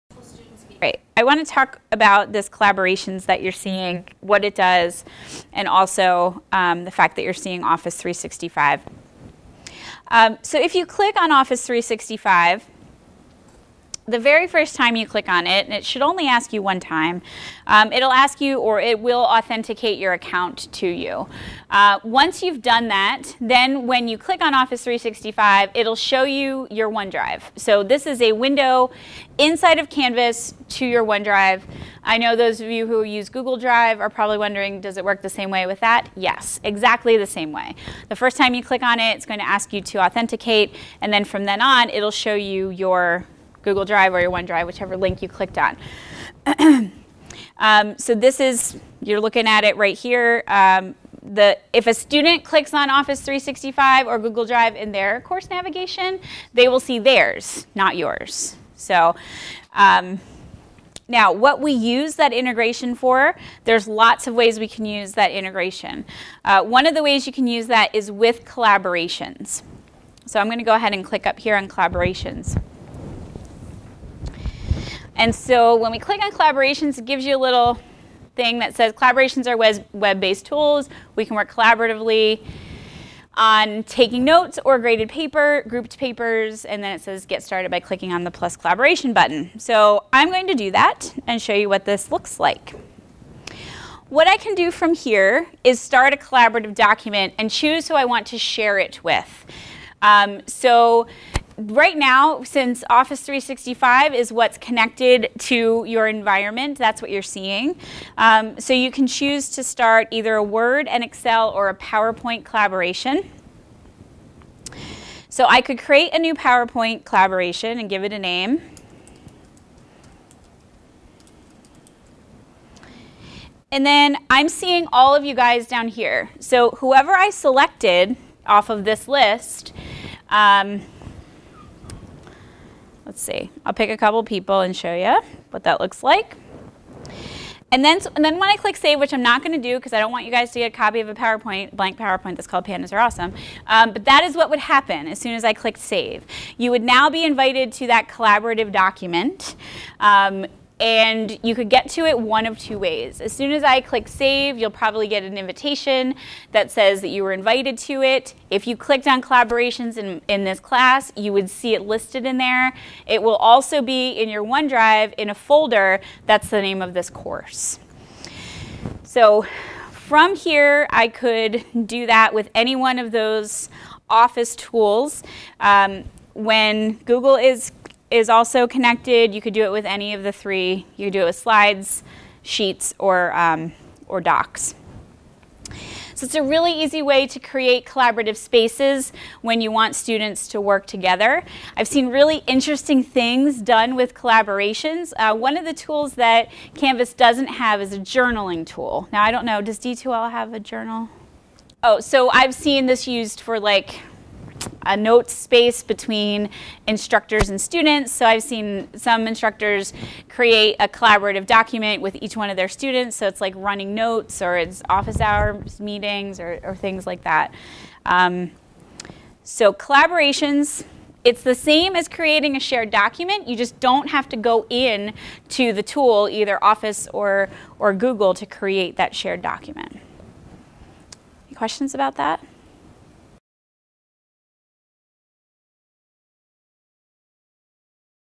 Canvas Collaborations: Office 365
Snippets from the UW-System Training sessions held during the Spring 2018 semester.